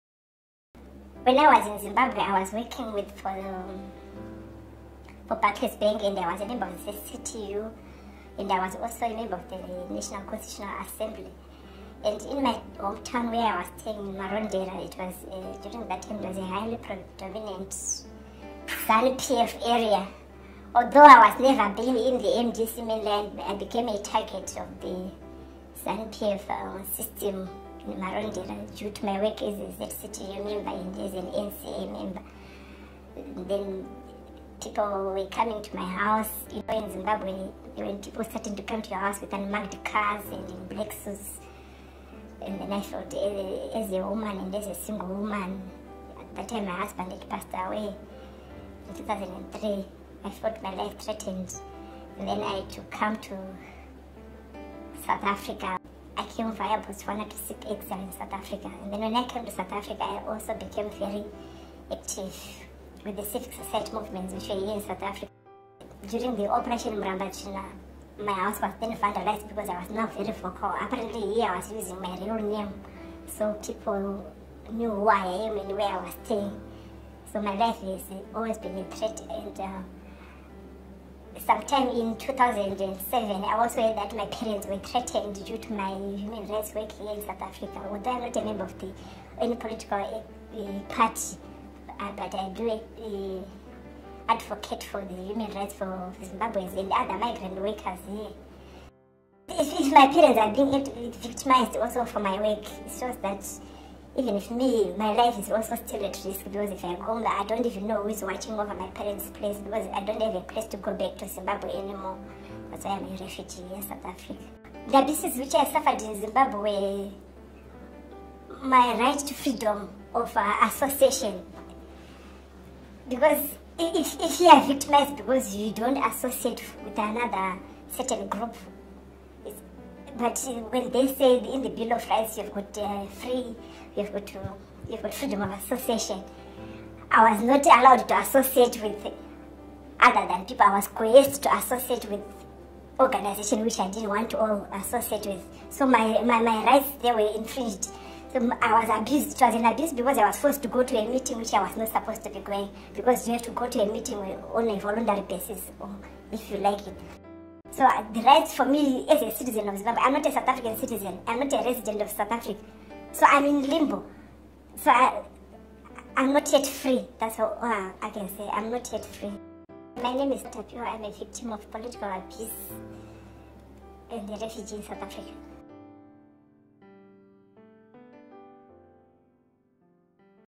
16 Days of Activism Against Gender Based Violence
Story #13 Tapiwa speaks of the political violence that she says has rendered her "homeless" and taken away her freedom